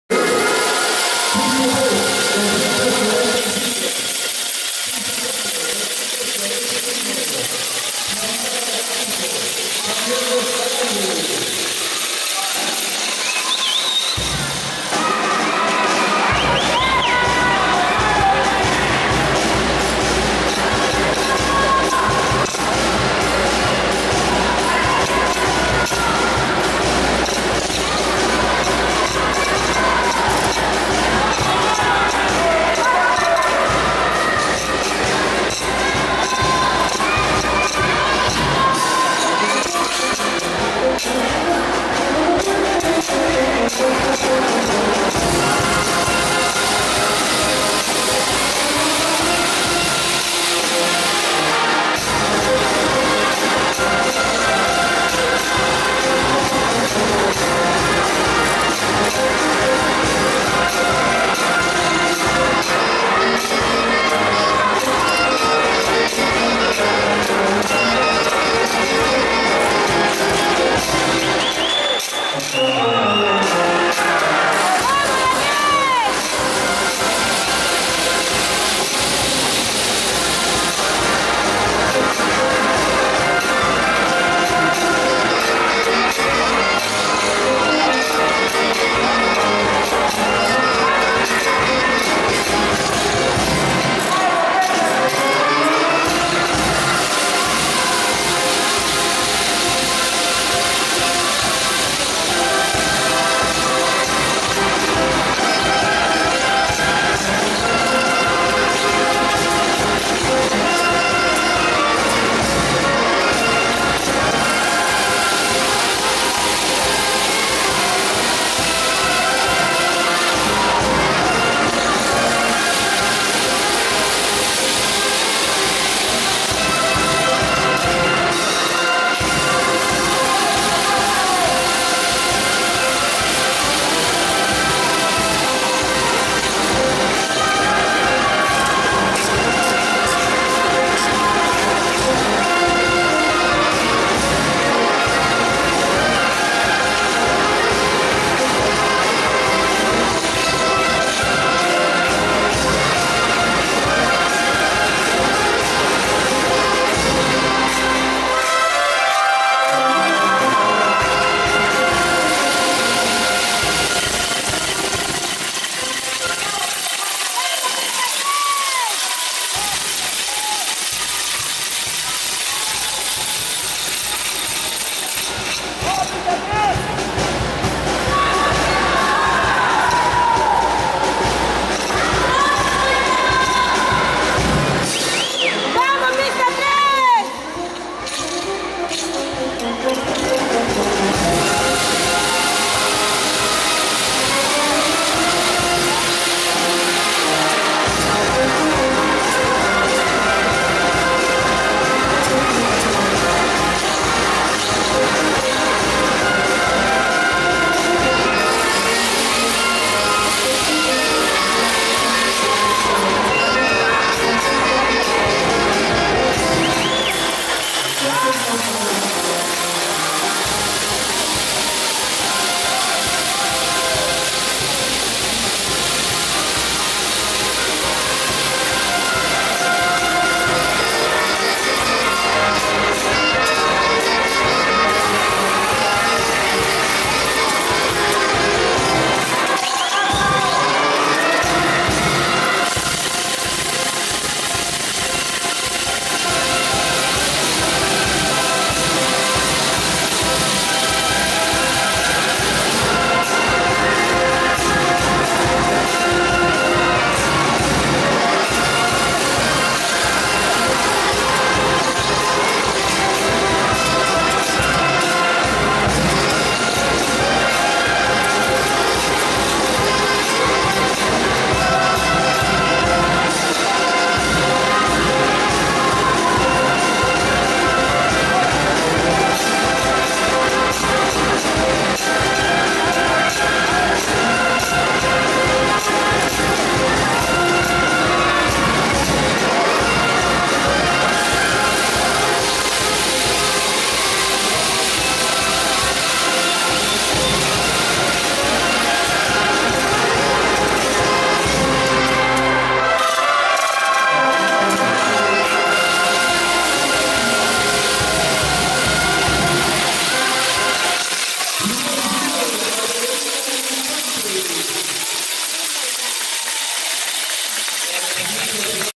"Marinera Norteña del Perú para el Mundo."
Daniel y Geraldine, son dos Niños prodigios que nacieron para brillar en la Marinera y eso les a valido ser Campeones y reconocidos en muchas partes. Cada 7 de Octubre se celebra el Día Nacional de la Marinera y si es Norteña es mucho mejor.